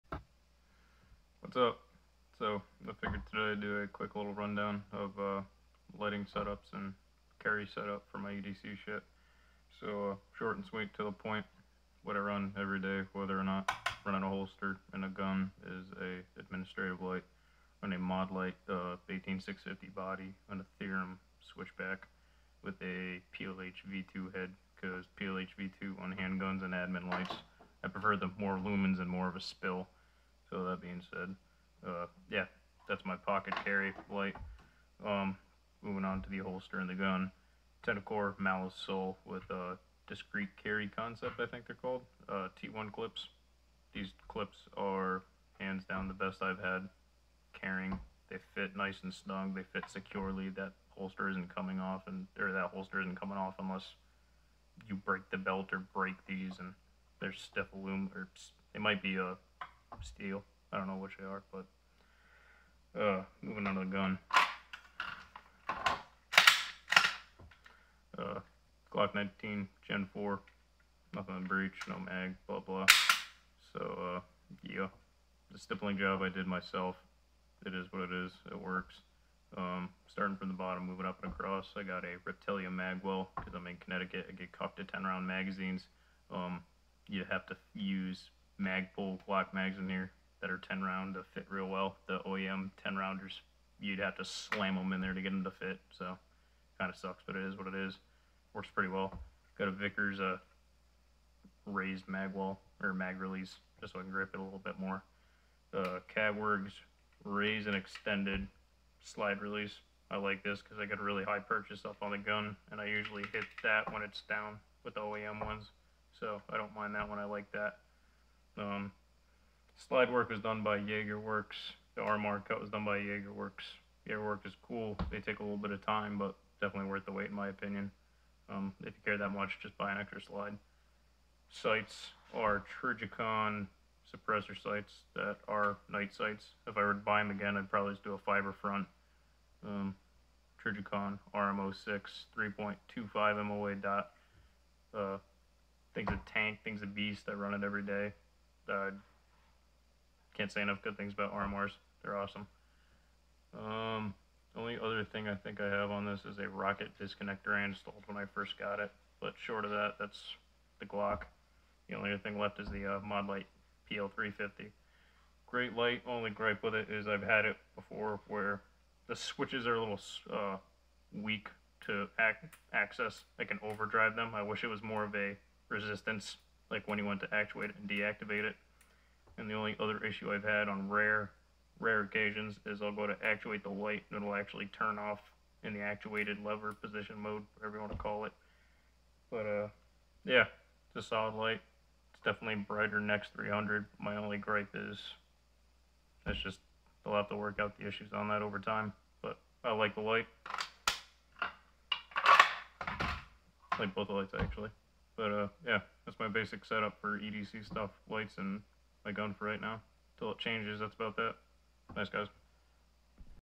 EDC Stuff
quick and basic rundown of what I use for lights and a carry piece.